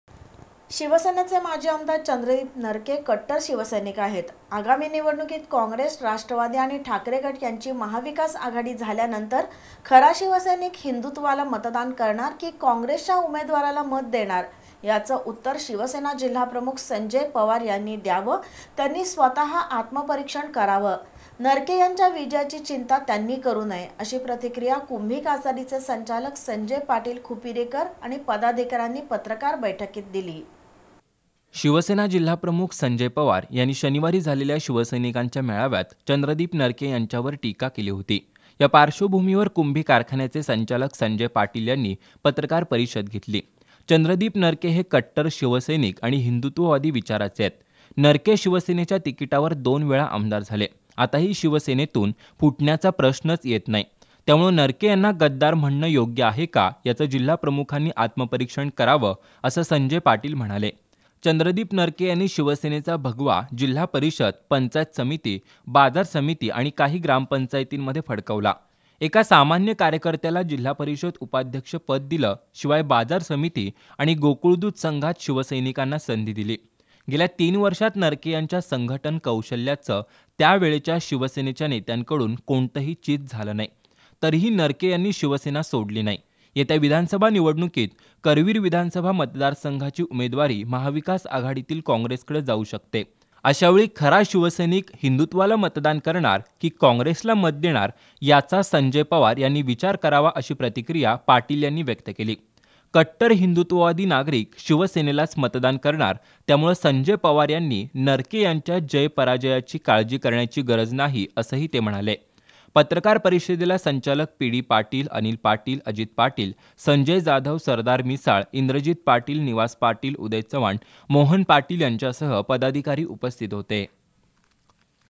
कट्टर शिवसैनिक हिंदुत्वाला की कॉंग्रेसला मतदान करणार याचं शिवसेना जिल्हाप्रमुखांनी उत्तर द्यावं
कट्टर शिवसैनिक हिंदुत्वाला की कॉंग्रेसला मतदान करणार याचं शिवसेना जिल्हाप्रमुखांनी उत्तर द्यावं, कुंभी कासारीचे संचालक संजय पाटील-खुपिरेकर यांची प्रतिक्रिया